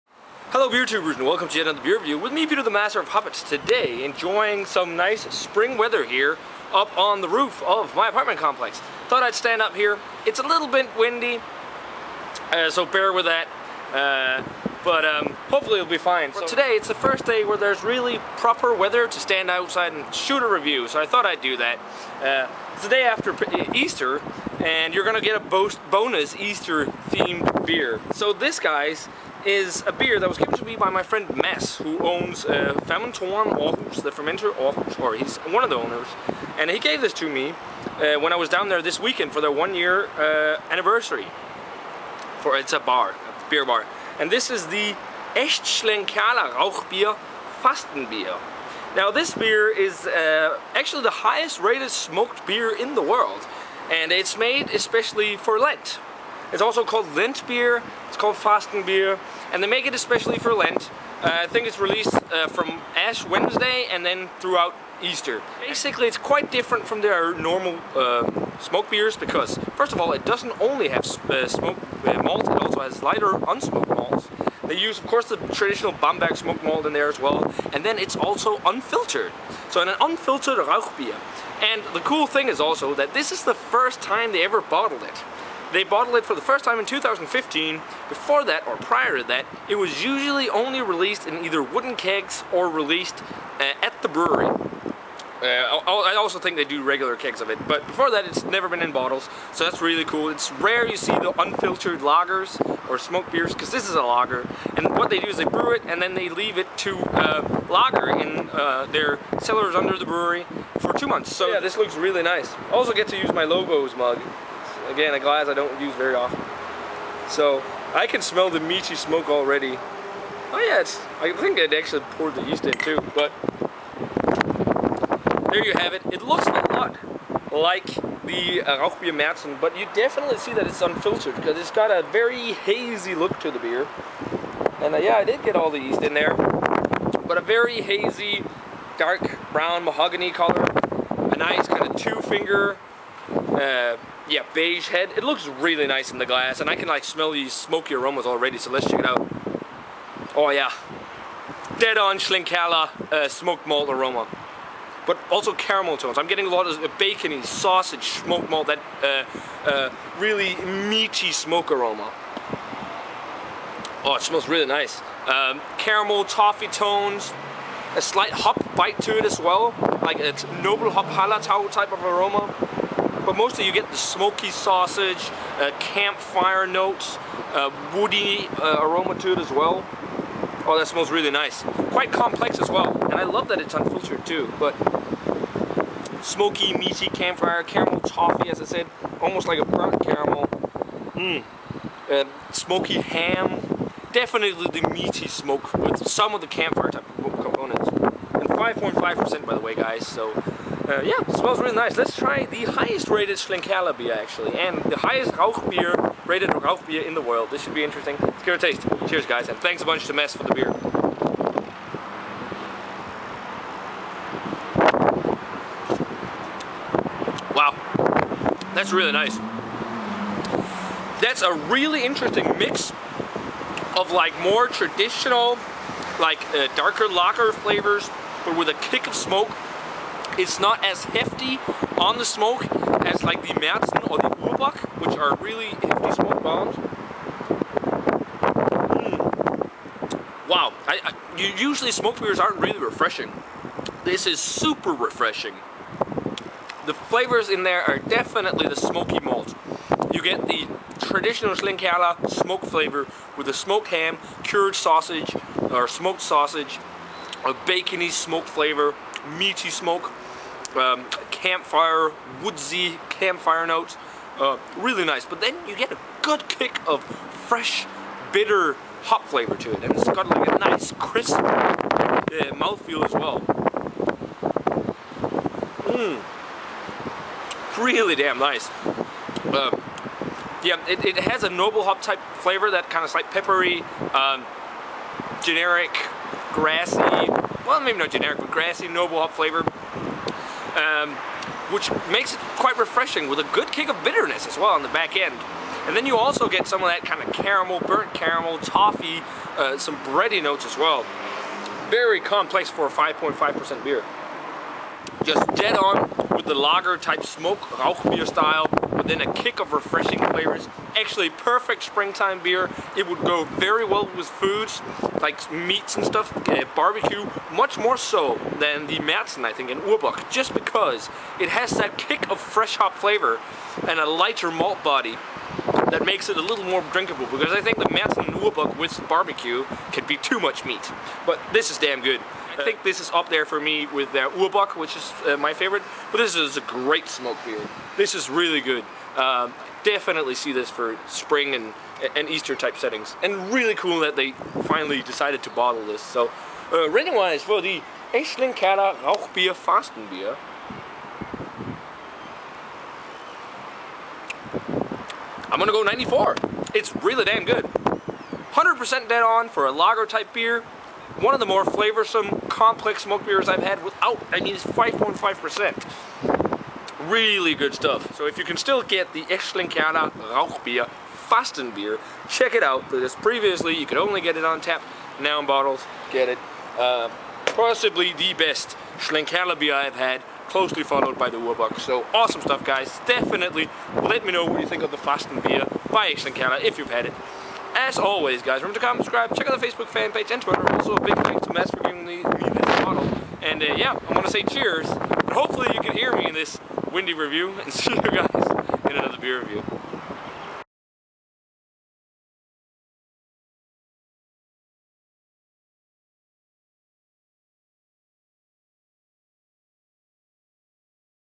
TMOH - Beer Review 1597#: Aecht Schlenkerla Fastenbier
Information About This Beer:

Brewery: Brauerei Heller
Beer: Aecht Schlenkerla Fastenbier
Style: Rauchbier/Smoked Beer
Location: Brewed by Brauerei Heller, in Bamberg, Bavaria, Germany.
ABV: 5,5%
Rating: 94/100